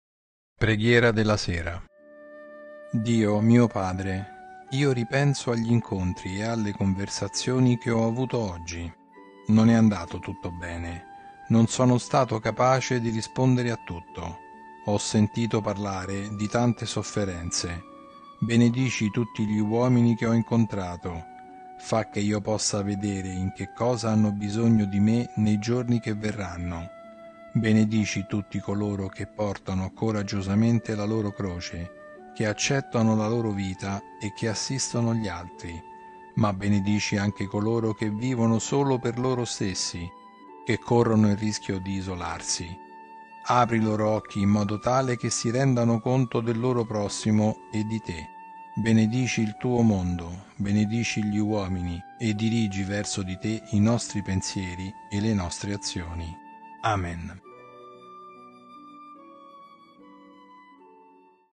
Preghiera della sera 17 Luglio 2024
Dio, mio Padre. Io ripenso agli incontri e alle conversazioni che ho avuto oggi. Non è andato tutto bene... Non sono stato capace di rispondere a tutto... Ho sentito parlare di tante sofferenze... Benedici tutti gli uomini che ho incontrato. Fa' che io possa vedere in che cosa hanno bisogno di me nei giorni che verranno. Benedici tutti coloro che portano coraggiosamente la loro croce, che accettano la loro vita e che assistono gli altri. Ma benedici anche coloro che vivono solo per loro stessi, che corrono il rischio di isolarsi. Apri i loro occhi in modo tale che si rendano conto del loro prossimo, e di Te. Benedici il Tuo mondo. Benedici gli uomini. E dirigi verso di Te i nostri pensieri e le nostre azioni, Amen.

Evening Prayer.
God, my Father. I think back on the meetings and conversations I had today. It did not go well... I have not been able to respond to everything. I have heard of so much suffering... Bless all the men I have met. Let me see what they need me for in the days to come. Bless all those who courageously bear their cross, who accept their lives and who assist others. But bless also those who live only for themselves, who run the risk of isolating themselves. Open their eyes in such a way that they become aware of their neighbour, and of You. Bless Your world. Bless mankind. And direct our thoughts and actions towards You, Amen.

Prière du soir.
Dieu, mon Père. Je repense aux réunions et aux conversations que j'ai eues aujourd'hui. Cela ne s'est pas bien passé... Je n'ai pas pu répondre à tout. J'ai entendu parler de tant de souffrances... Bénissez tous les hommes que j'ai rencontrés. Laissez-moi voir ce qu'ils attendent de moi dans les jours à venir. Bénissez tous ceux qui portent courageusement leur croix, qui acceptent leur vie et qui aident les autres. Mais bénis aussi ceux qui ne vivent que pour eux-mêmes, qui risquent de s'isoler. Ouvre leurs yeux pour qu'ils prennent conscience de leur prochain et de Toi. Bénis ton monde. Bénis l'humanité. Et oriente nos pensées et nos actions vers Toi, Amen.

Oración de la tarde.
Dios, Padre mío. Pienso en las reuniones y conversaciones que he tenido hoy. No han ido bien... No he podido responder a todo. He oído hablar de tanto sufrimiento... Bendice a todos los hombres que he conocido. Déjame ver para qué me necesitan en los próximos días. Bendice a todos los que llevan valientemente su cruz, que aceptan su vida y que ayudan a los demás. Pero bendice también a los que viven sólo para sí mismos, a los que corren el riesgo de aislarse. Abre sus ojos para que tomen conciencia de su prójimo y de Ti. Bendice Tu mundo. Bendice a la humanidad. Y dirige nuestros pensamientos y acciones hacia Ti, Amén.

Deus, meu Pai. Penso nos encontros e nas conversas que tive hoje. Não correu bem... Não pude responder a tudo. Ouvi falar de tanto sofrimento... Abençoados sejam todos os homens que encontrei. Deixai-me ver o que precisam de mim nos próximos dias. Abençoa todos aqueles que carregam corajosamente a sua cruz, que aceitam a sua vida e que ajudam os outros. Mas abençoa também aqueles que vivem apenas para si próprios, que correm o risco de se isolarem. Abri os seus olhos para que tomem consciência do seu próximo e de Vós. Abençoa o Teu mundo. Abençoa a humanidade. E orienta os nossos pensamentos e as nossas acções para Ti, Amém.